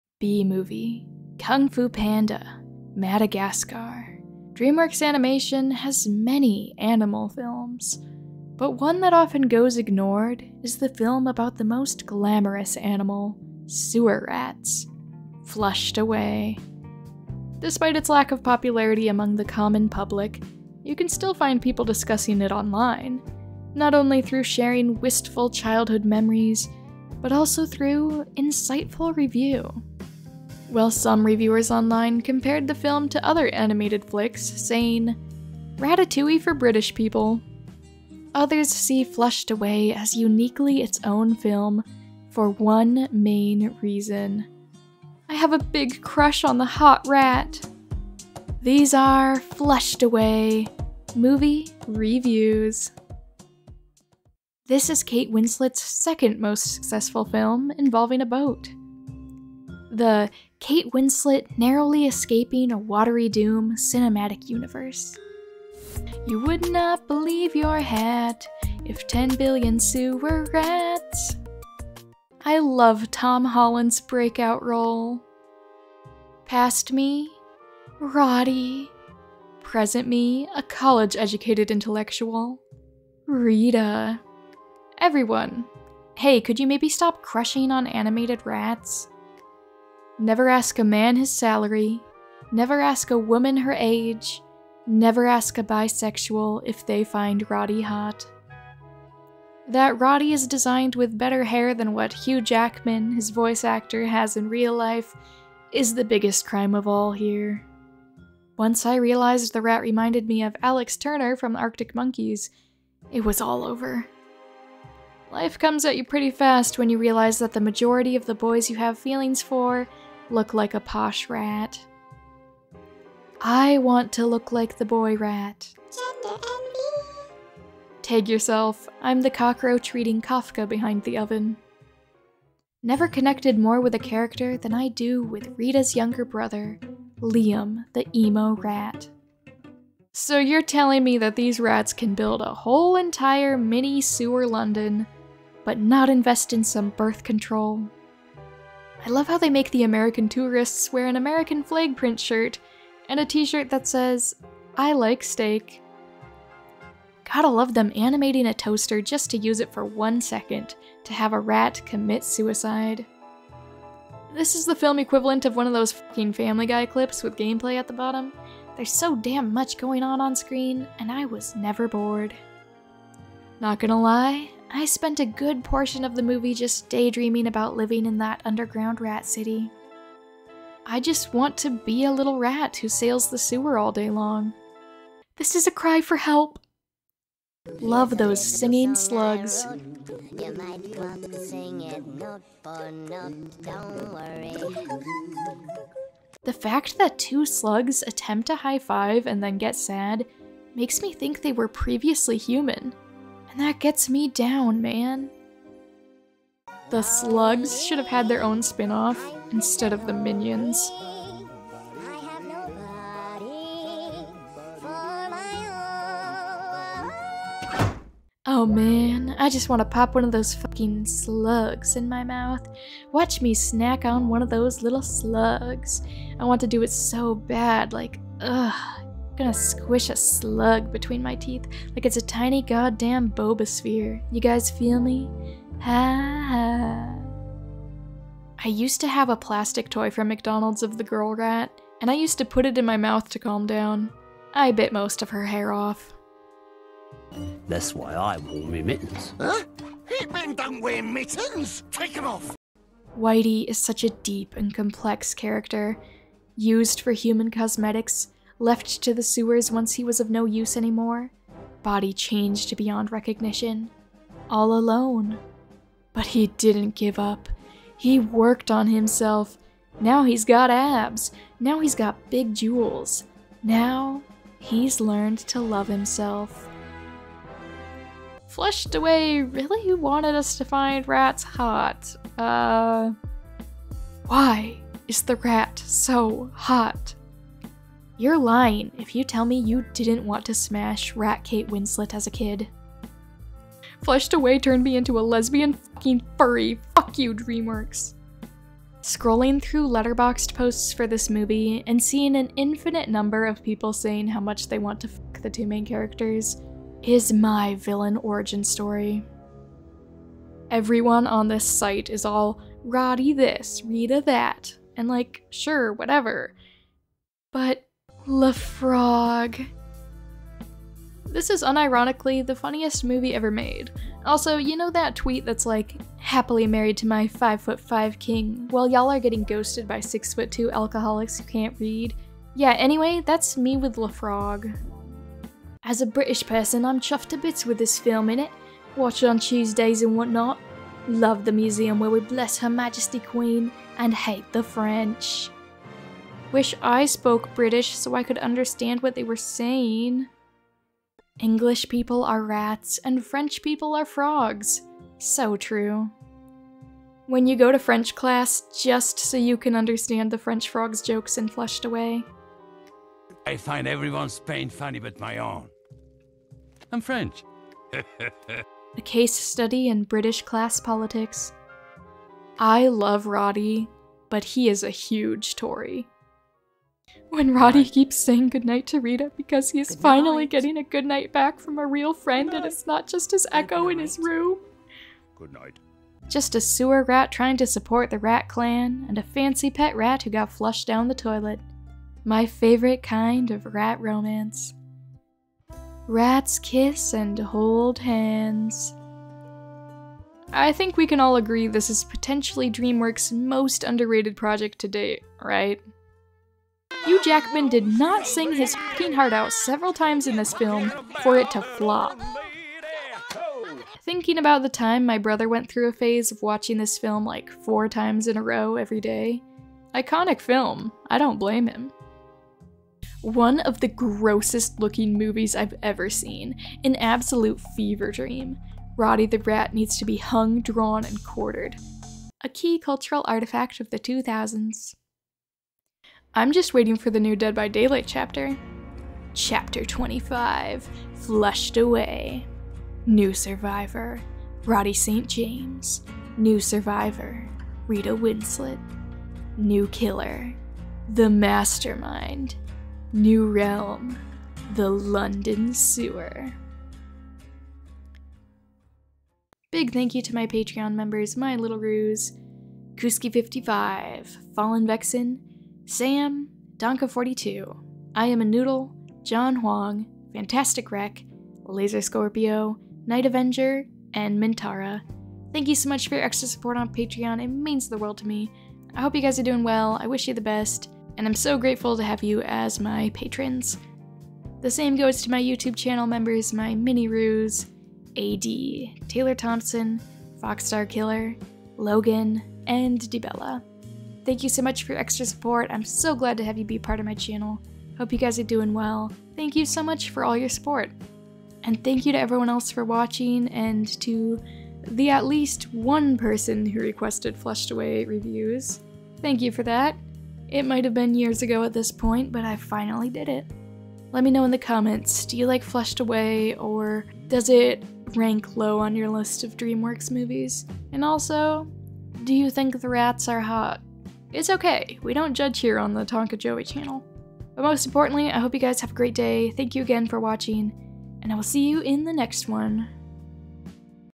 "Flushed Away" Made Everyone a Furry
Flushed Away is not one of Dreamworks Animations's most popular films about animals, but it might have had the most power when it came to turning people into furries. So, let's read some humorous reviews of the movie from the site Letterboxd to hear about everyone's crushes on the rats Roddy and Rita, as well as comments on plenty of other things! Enjoy!

0:00 Intro
1:16 Roddy!!
3:07 Singing Slugs
4:38 Rita!!
5:35 British Rats
6:17 Rat Romance
7:15 DBD